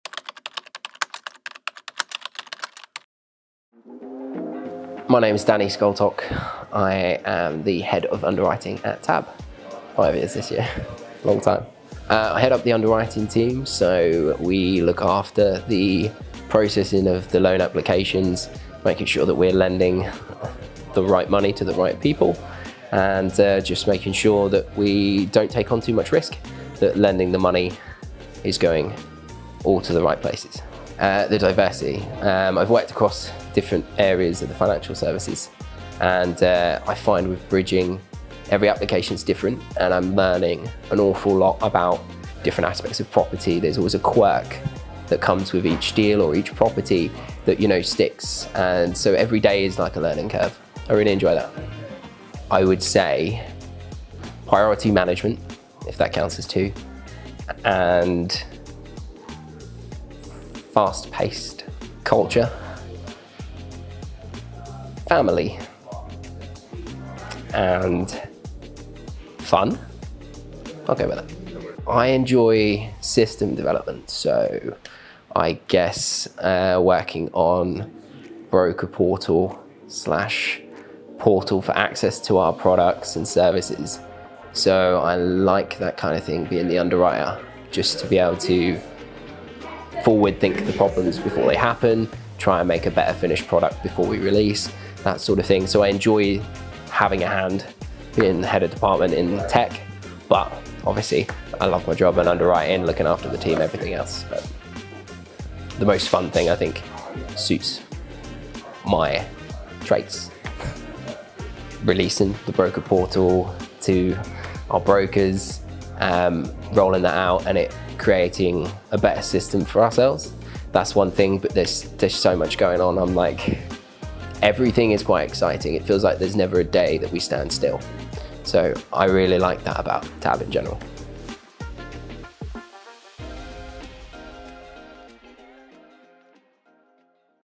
A Day in the life... Danny Scoltock, Head of Underwriting
Get to know our head of underwriting, Danny.